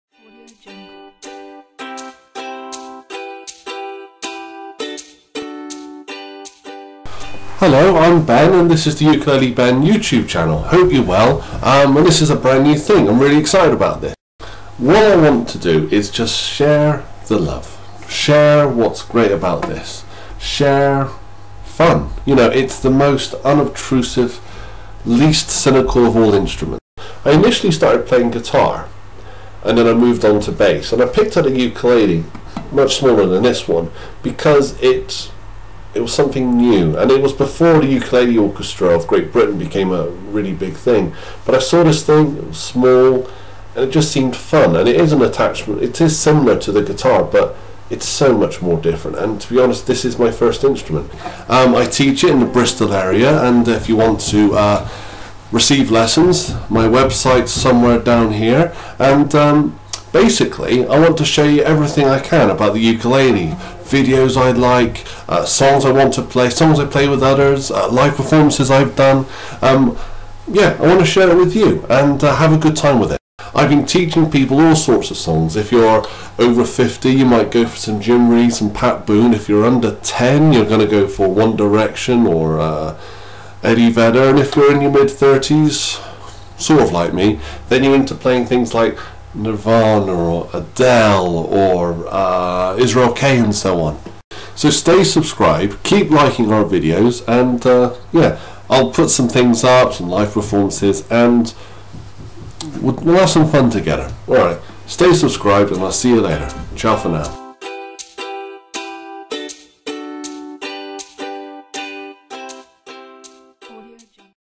Fun with Ukulele Ben
This is my first video.  This is who I am and what this channel is all about.  Enjoy!